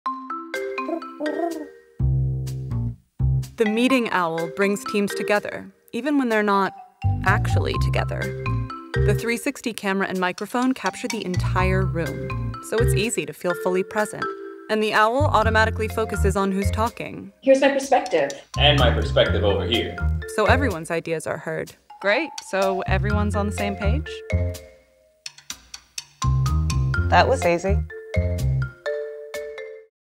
Meet the Meeting Owl
Welcome to Owl Labs: Where collaboration meets perspective. Meeting the Meeting Owl Pro, our flagship product that is reinventing the way the world collaborates.